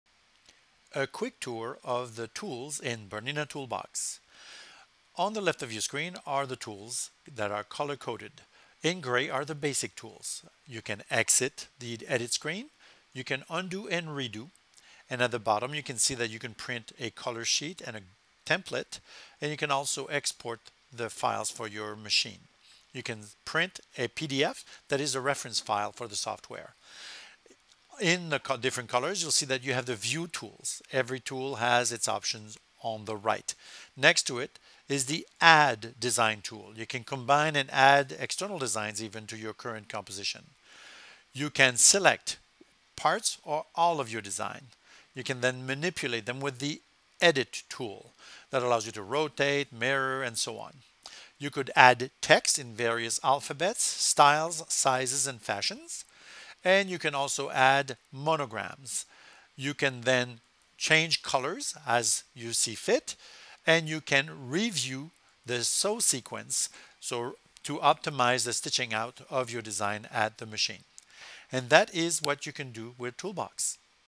BERNINA Toolbox Tutorial Part 4: Review of Tools
BERNINA Toolbox offers a variety of tools to edit, customize and add lettering and monograms to your embroidery. This video will help you determine what tool(s) to use for your various projects.